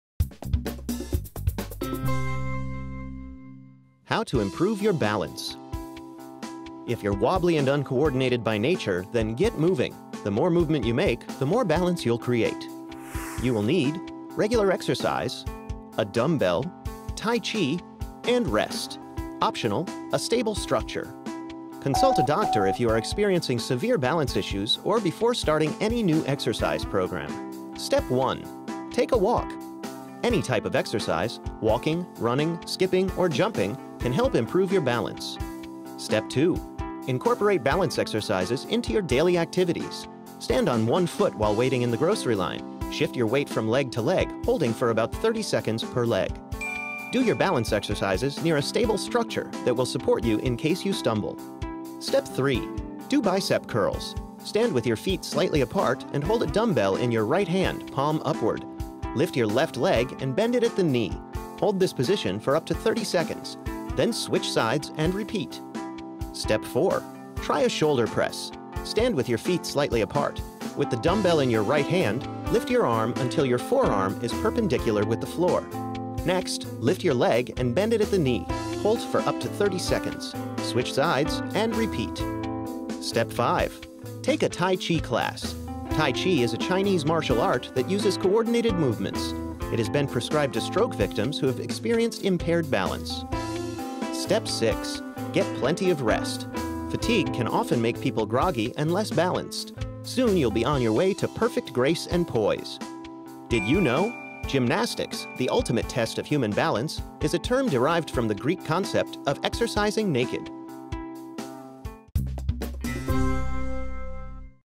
How to Improve Your Balance
If you're wobbly and uncoordinated by nature, then get moving – the more movement you make, the more balance you’ll create.

Warning
Consult a doctor if you experience severe balance issues or before starting a new exercise program.

Step 1: Get moving
Take a walk. Any type of exercise -- walking, running, skipping, or jumping -- can help improve your balance.

Step 2: Try balance exercise daily
Incorporate balance exercises into your daily activities. Stand on one foot while waiting in the grocery line; shift your weight from leg to leg, holding for about 30 seconds per leg.

Tip
Do your balance exercises near a stable structure that will support you in case you stumble.

Step 3: Do bicep curls
Do bicep curls. Stand with your feet slightly apart and hold a dumbbell in your right hand, palm upward. Lift your left leg and bend it at the knee. Hold this position for up to 30 seconds. Then switch sides and repeat.

Step 4: Do shoulder presses
Try a shoulder press. Stand with your feet slightly apart. With the dumbbell in your right hand, lift your arm until your forearm is perpendicular with the floor. Next lift your left leg and bend it at the knee. Hold for up to 30 seconds. Switch sides and repeat.

Step 5: Try tai chi
Take a tai chi class. Tai chi is a Chinese martial art that uses coordinated movements. It has been prescribed to stroke victims who have experienced impaired balance.

Step 6: Get rest
Get plenty of rest. Fatigue can often make people groggy and less balanced. Soon you’ll be on your way to perfect grace and poise.

Did you know? Gymnastics, the ultimate test of human balance, is a term derived from the Greek concept of exercising naked.